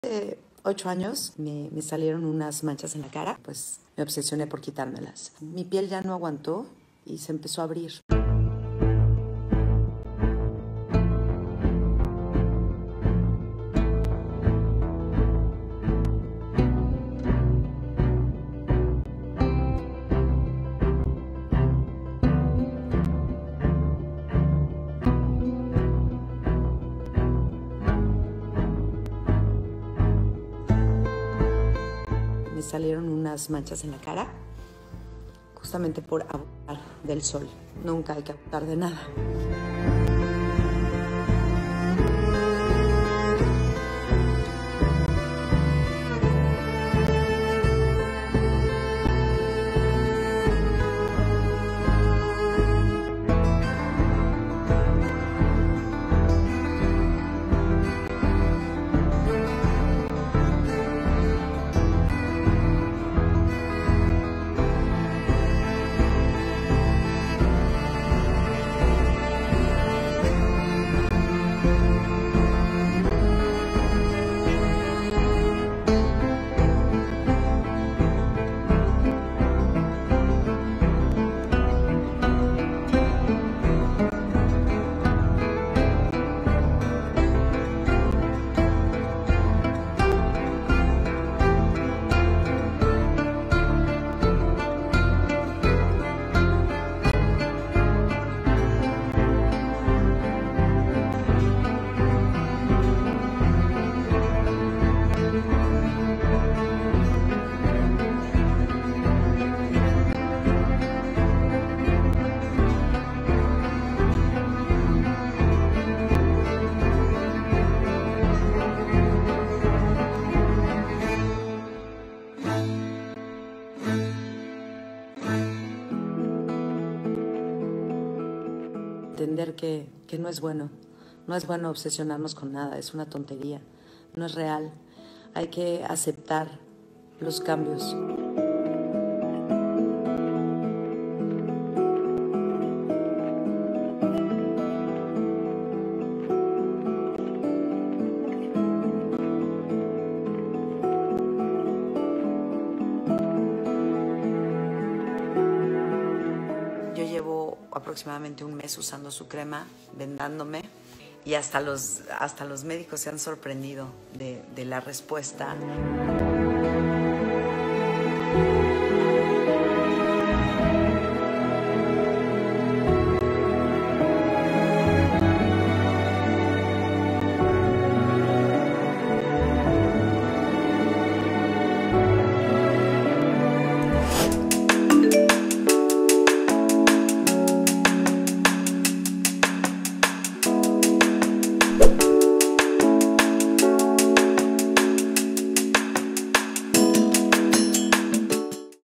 El rostro de Fabiola Campomanes se dañó por tratamiento estético
“Me cegue por lucir perfecta, pero mi piel no aguanto y se abrió” Fabiola Campomanes tenía un cutis espectacular, siempre procuraba cuidárselo al máximo, sin embargo, excederse de pasar horas bajo el sol, provocó que le salieran manchas horribles en el rostro y terminara tomando la peor decisión de todas al querer quitárselas con numerosos tratamientos estéticos, que no salieron nada bien y la dejaron irreconocible.